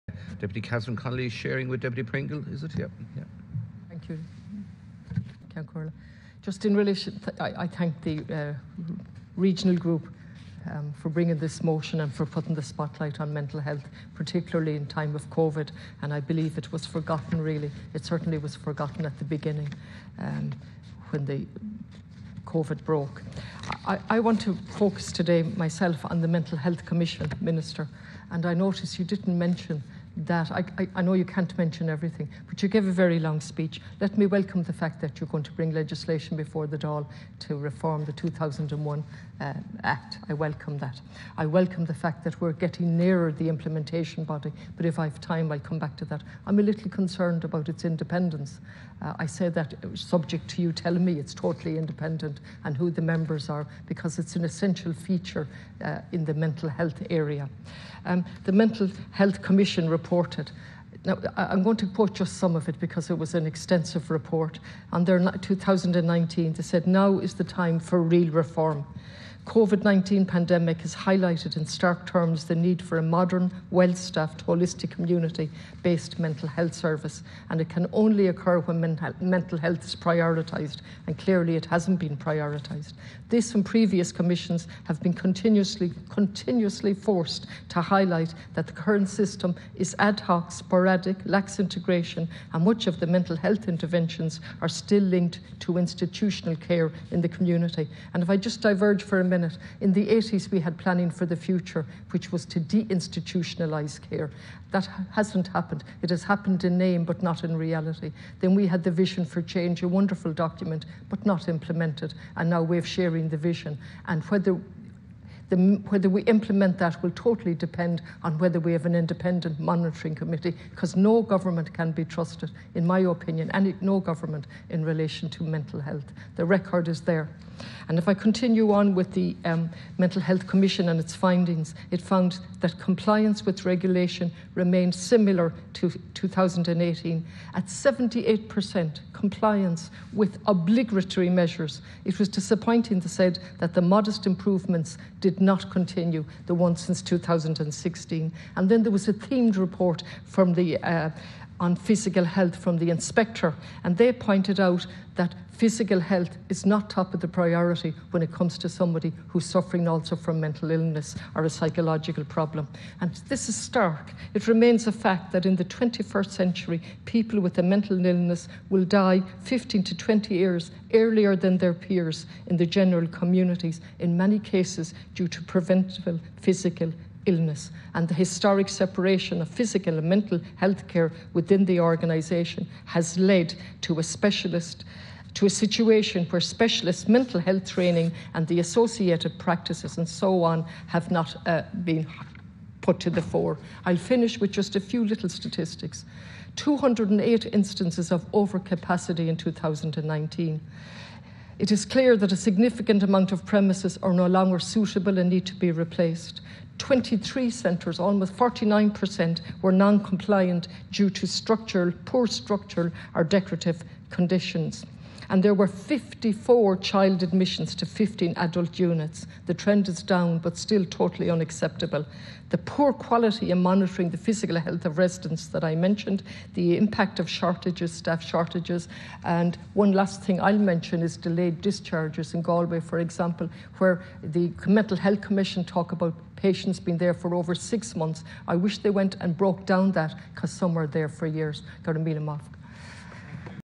Catherine Connolly TD speaking on the Motion on Mental Health
Covid-19 has highlighted the importance of Mental Health and the necessity of having adequate Mental Health Services. Legislation will be brought forward to refrom The Mental Health Act 2001, however we should have an independent monitoring committee to ensure the implementation of 'Sharing the Vision: A Mental Health Policy for Everyone'.

9.12.20